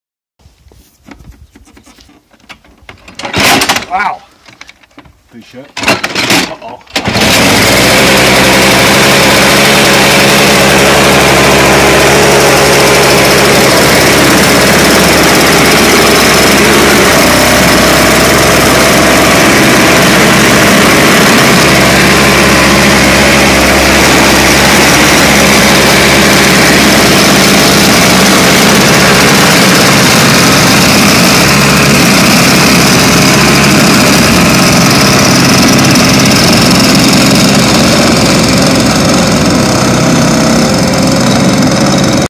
1984 Toro S-620 Snow Thrower
Got this little gem from my father-in-law. It sat outside in the elements for a long time. Maybe two years or so. I picked it up thinking that I could get it running. Sure enough! A new spark plug and rebuilding the carb took care of any issues he was having getting it started. Here she is! (And almost as old as I am!) She made it through about 1.5 feet (thick) of snow and only smoked a little extra. May have bogged a bit, but not really enough to notice.
I'm ready for more snow now!